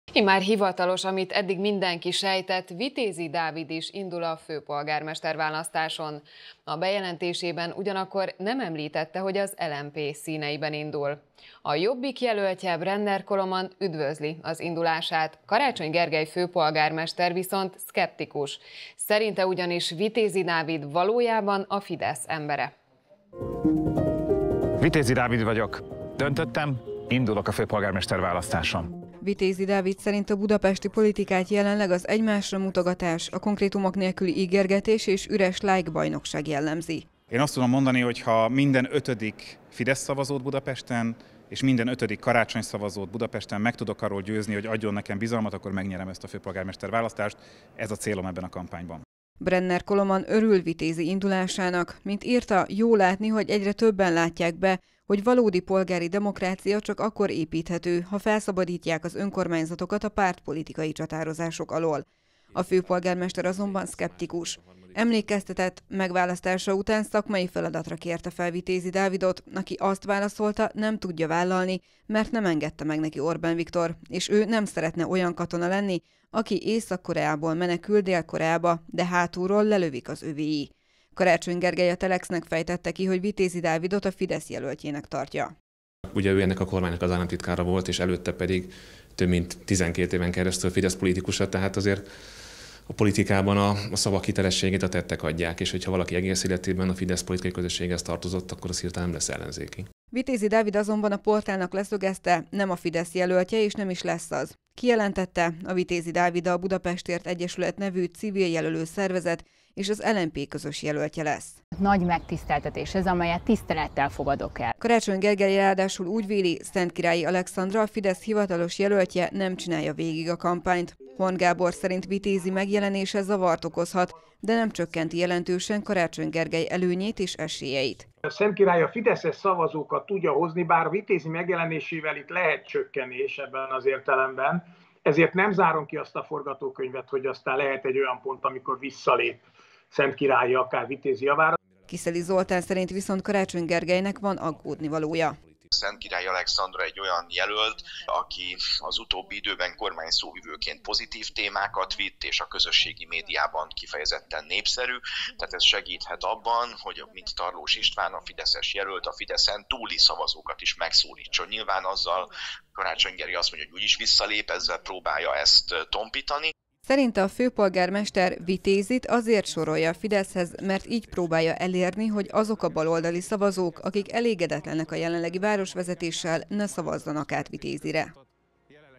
Vitézy: Döntöttem, indulok a főpolgármester-választáson
A bejelentésében ugyanakkor nem említette, hogy az LMP színeiben indul. A Jobbik jelöltje, Brenner Koloman üdvözli az indulását, Karácsony Gergely főpolgármester viszont szkeptikus. Szerinte ugyanis Vitézy Dávid valójában a Fidesz embere.